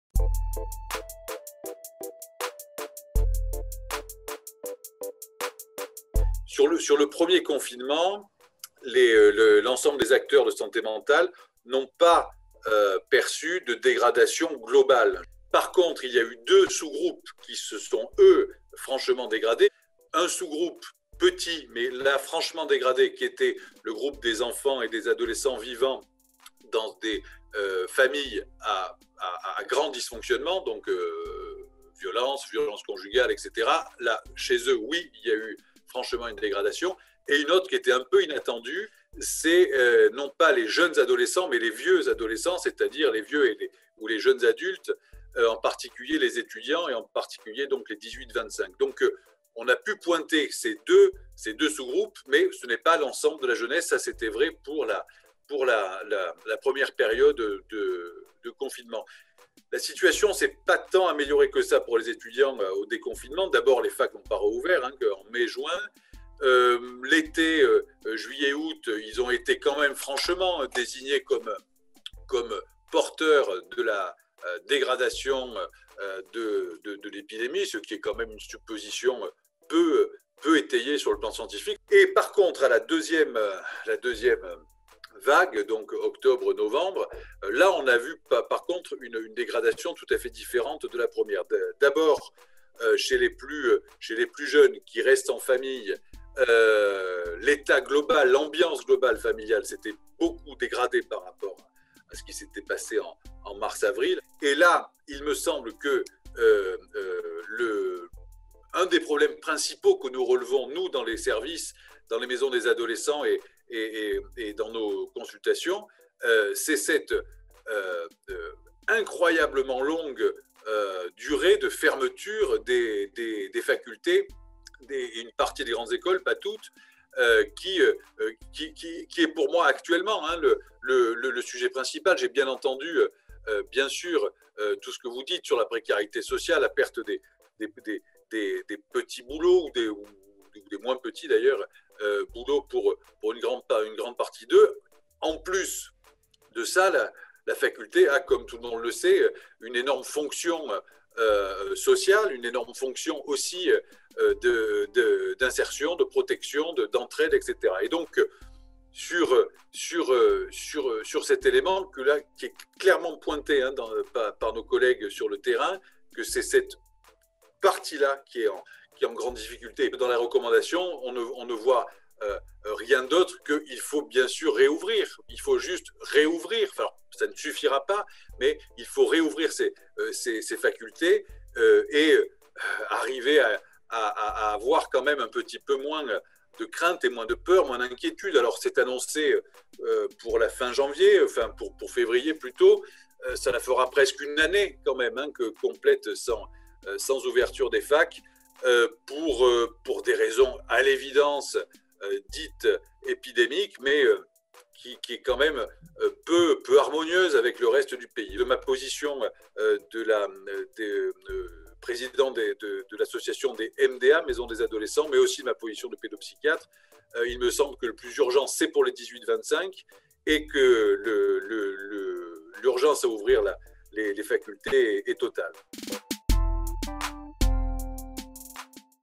Réaction de la MDA à la déclaration du CESE
Réaction de l'association des maisons des adolescents  à la déclaration "Jeunes, le devoir d'avenir", adoptée par le Bureau du CESE.
Pr. Bronsard, Président de l'association des maisons des adolescents.